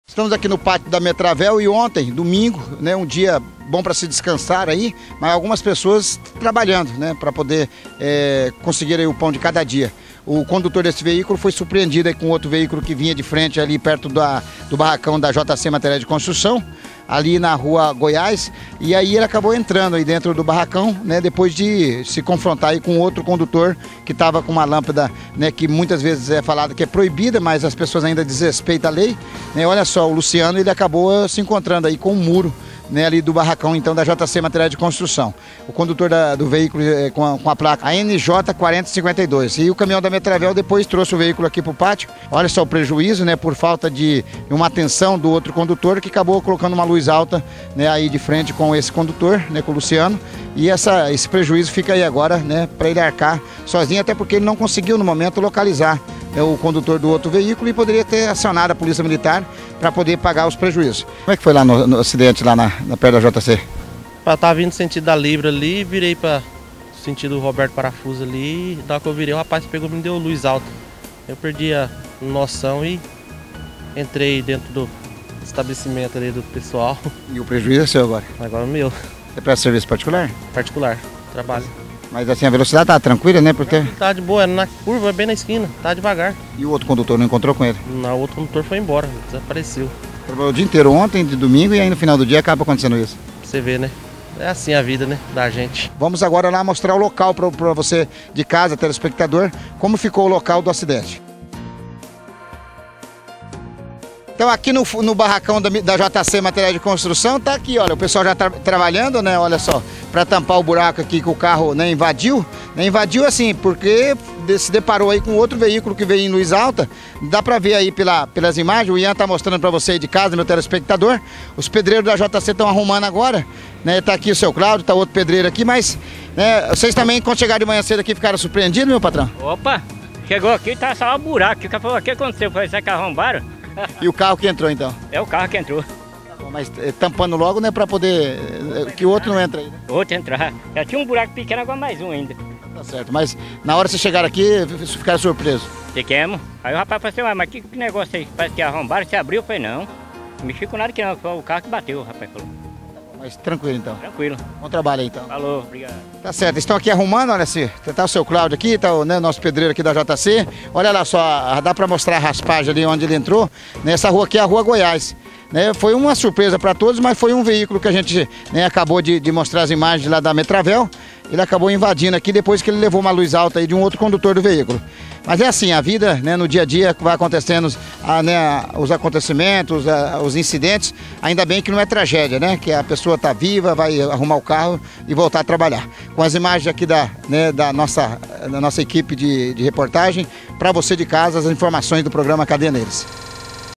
Veículo invade depósito da JC materiais de construções
Reportagem de Adeilson Correa Imagens Yan Montanari